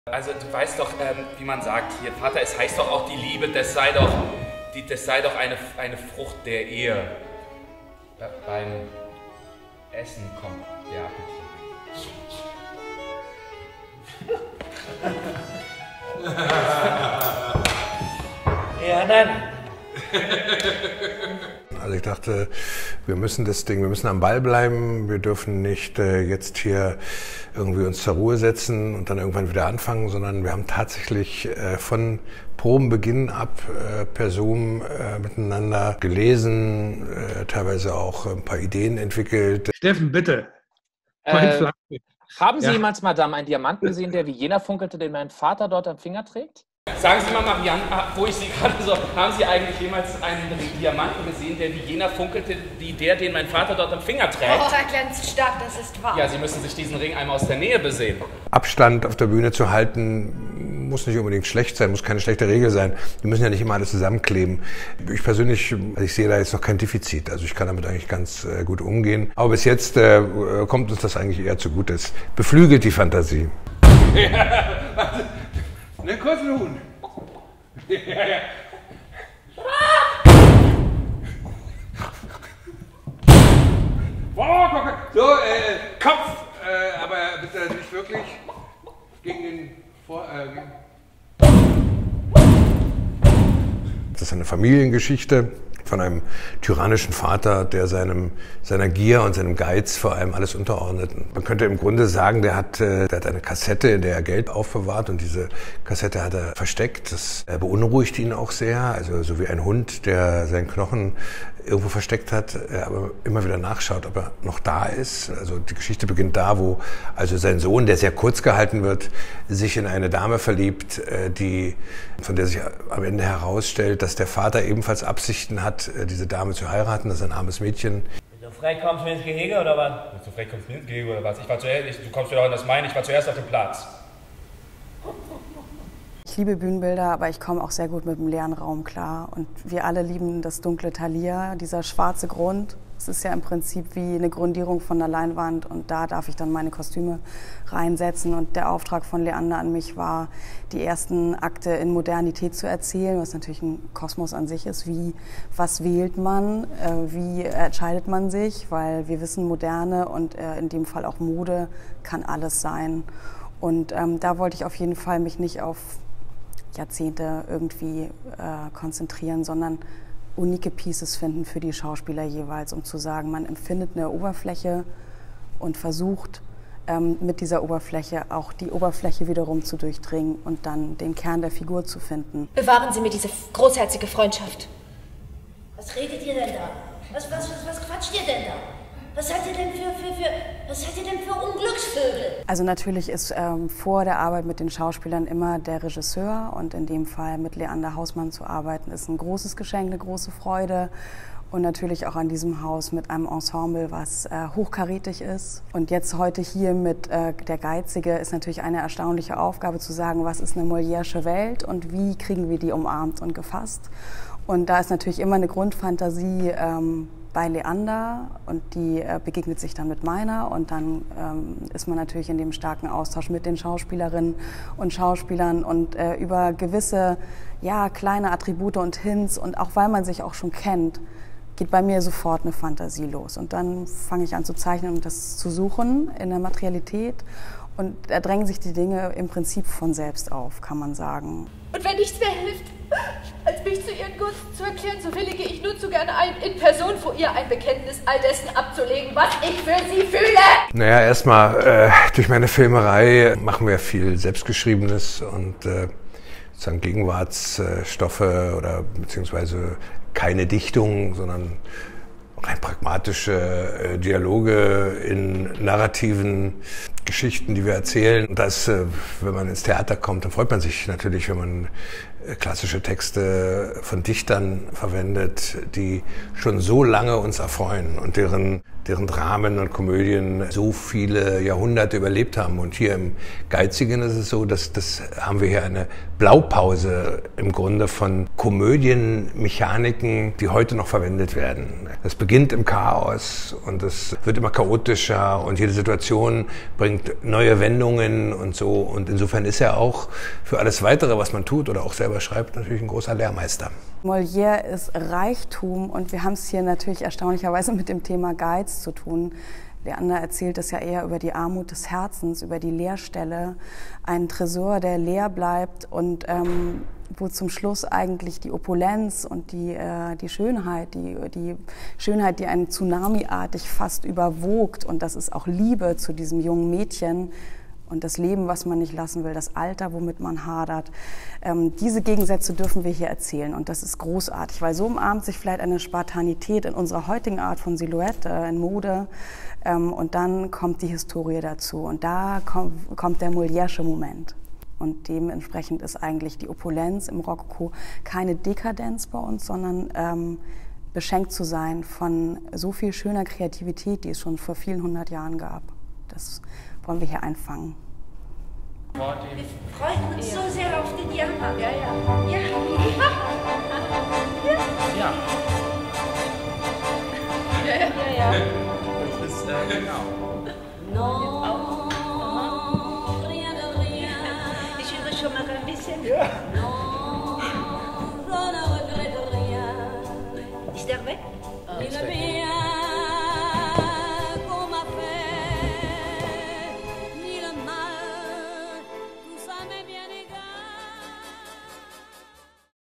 Der Geizige - Making Of
Der Geizige
oder Die Schule der Lügner von Molière

Regie Leander Haußmann
Bühne Peter Schubert
Kostüme Janina Brinkmann
Dramaturgie Susanne Meister
Licht Jan Haas

Mit
Jens Harzer (Harpagon), Steffen Siegmund (Cléante), Rosa Thormeyer (Mariane), Pascal Houdus (Valère), Toini Ruhnke (Elise), Marina Galic (Frosine), Sebastian Zimmler (La Flèche / Anselme), Tim Porath (Maître Jacques / Maître Simon)

Making Of Miriam Endrulat